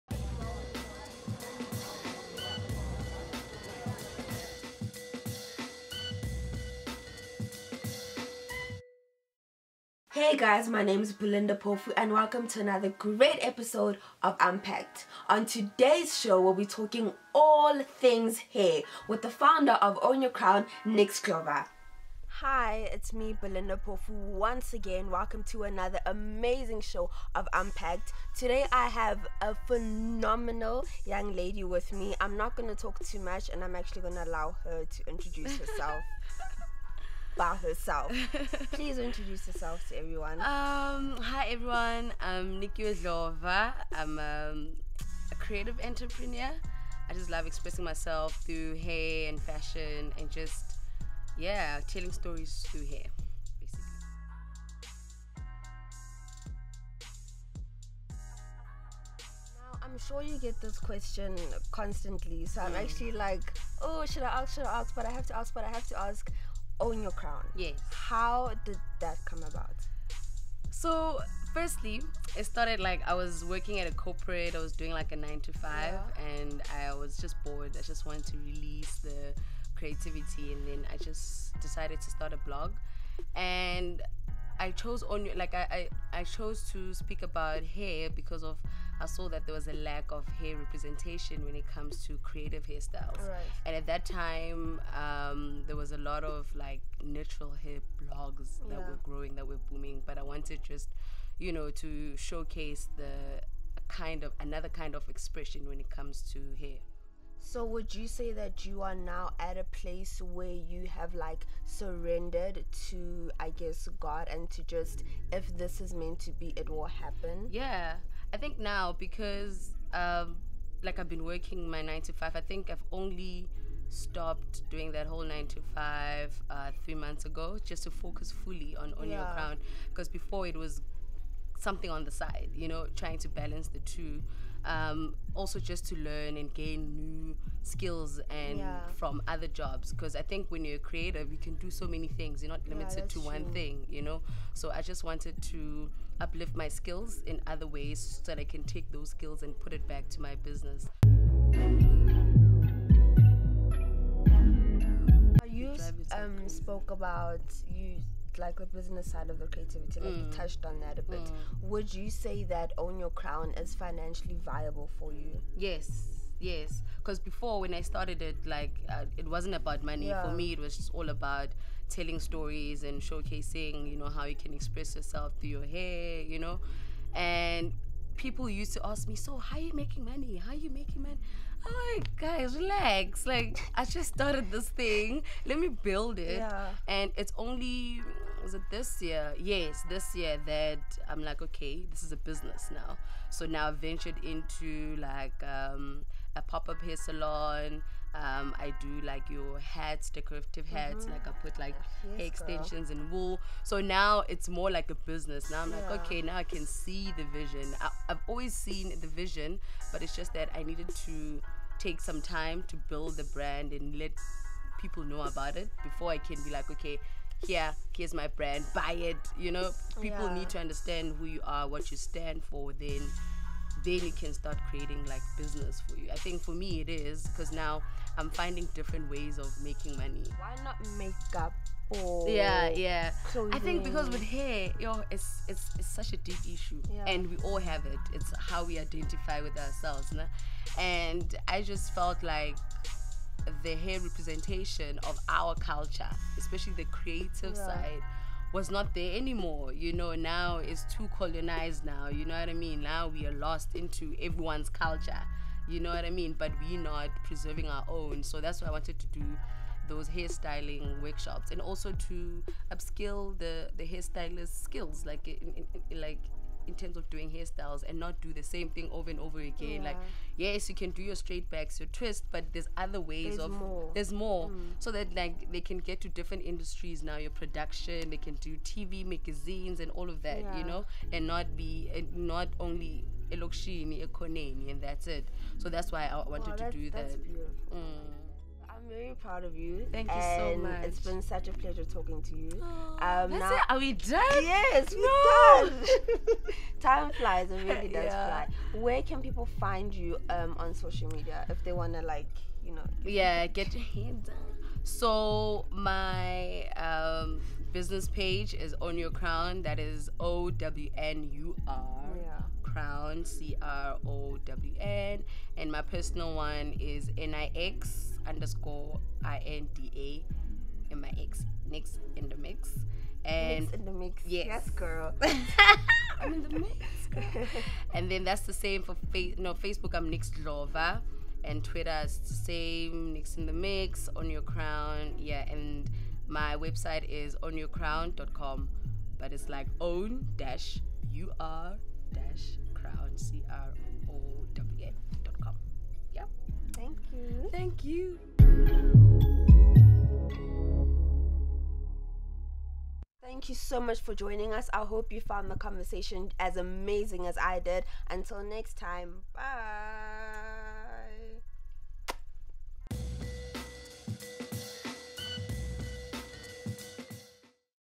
UNPAC'D Presents Nix Dlova: Unpacking all things hair, fashion and creative entrepreneurship
Nix Dlova as Nikiwe is best known is a fashion, hair and cycling enthusiast from Johannesburg. Nix has a blog called Own Ur Crown where she has reinvented they way we view our  Betty hairstyles that we used to do in Primary school. Nikiwe’s hair changes everything I see her and I love that no hairstyle of hers is ever the same and she is not shy to use bright colours.